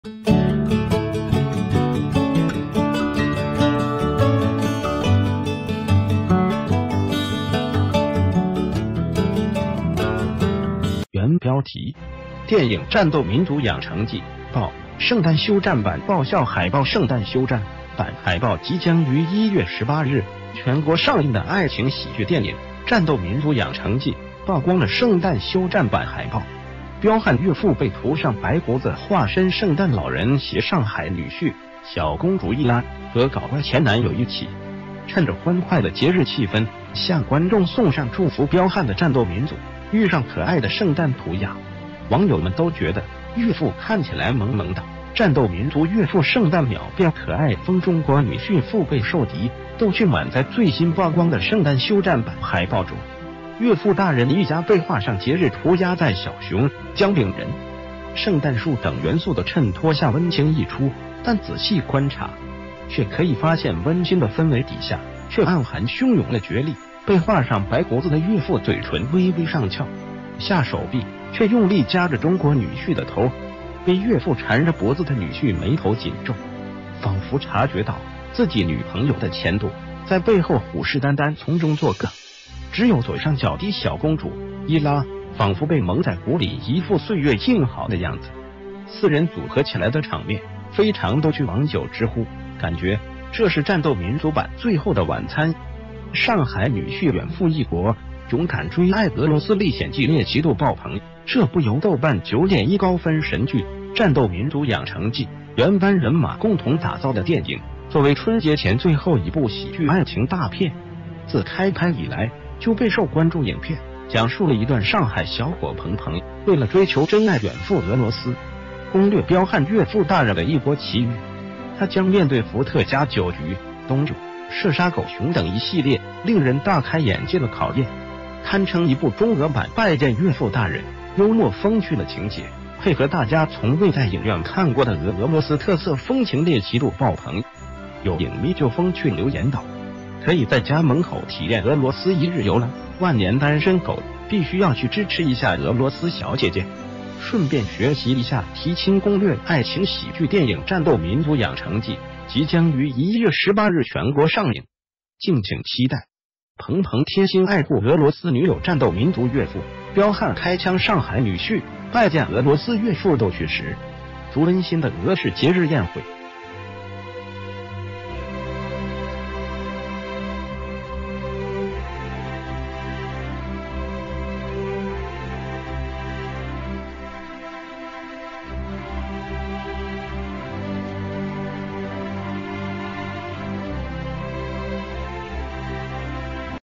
電影《戰斗民族養成記》曝“圣誕休戰”版爆笑海報
谢谢！

      原標題：電影《戰斗民族養成記》曝“圣誕休戰”版爆笑海報

“圣誕休戰”版海報
即將于1月18日全國上映的愛情喜劇電影《戰斗民族養成記》曝光了“圣誕休戰”版海報，彪悍岳父被涂上白胡子化身圣誕老人，攜上海女婿、“小公主”伊拉和搞怪前男友一起，襯著歡快的節日氣氛向觀眾送上祝福。彪悍的戰斗民族遇上可愛的圣誕涂鴉，網友們都覺得“岳父看起來萌萌噠”。
戰斗民族岳父圣誕秒變可愛風 中國女婿腹背受敵逗趣滿載
最新曝光的“圣誕休戰”版海報中，岳父大人一家被畫上節日涂鴉，在小熊、姜餅人，圣誕樹等元素的襯托下，溫情溢出。但仔細觀察，卻可以發現溫馨的氛圍底下卻暗含洶涌的角力：被畫上白胡子的岳父嘴唇微微上翹，下手臂卻用力夾著中國女婿的頭；被岳父纏著脖子的女婿眉頭緊皺，仿佛察覺到自己女朋友的前度在背后虎視眈眈從中作梗；只有左上角的“小公主”伊拉仿佛被蒙在鼓里，一副歲月靜好的樣子，4人組合起來的場面非常逗趣，網友直呼“感覺這是戰斗民族版最后的晚餐！”。
上海女婿遠赴異國勇敢追愛 俄羅斯歷險記獵奇度爆棚
這部由豆瓣9.1高分神劇《戰斗民族養成記》原班人馬共同打造的電影，作為春節前最后一部喜劇愛情大片，自開拍以來就備受關注，影片講述了一段上海小伙彭鵬為了追求真愛遠赴俄羅斯，攻略彪悍岳父大人的異國奇遇。他將面對伏特加酒局、冬泳、射殺狗熊等一系列令人大開眼界的考驗，堪稱一部中俄版《拜見岳父大人》。幽默風趣的情節配合大家從未在影院看過的俄羅斯特色風情，獵奇度爆棚。有影迷就風趣留言道：“可以在家門口體驗俄羅斯一日游啦”“萬年單身狗必須要去支持一下俄羅斯小姐姐，順便學習一下提親攻略”。
愛情喜劇電影《戰斗民族養成記》即將于1月18日全國上映，敬請期待！

彭鵬貼心愛護俄羅斯女友

戰斗民族岳父彪悍開槍

上海女婿拜見俄羅斯岳父逗趣十足

溫馨的俄式節日宴會